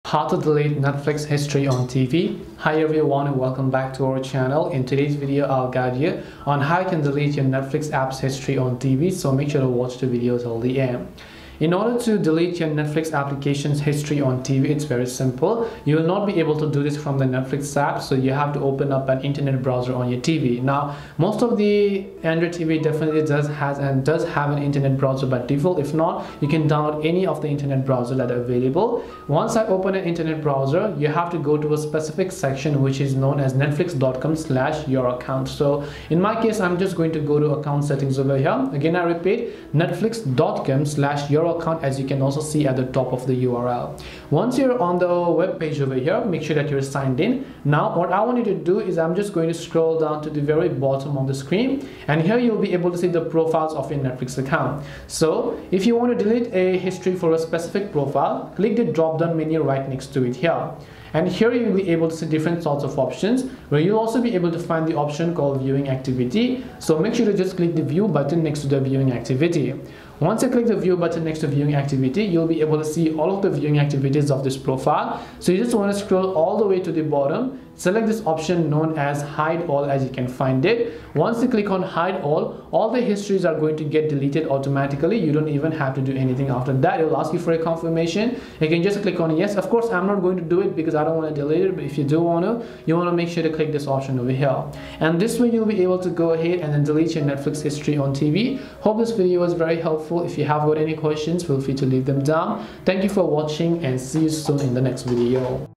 How to Delete Netflix History on TV !
In this tutorial video, I will quickly guide you on how you can delete your history on Netflix on TV. So, make sure to watch this video till the end. We try to be responsive to as many questions as possible.

► Chapters:
0:00 Introduction
0:11 General Info
0:44 Delete Netflix History
1:35 Conclusion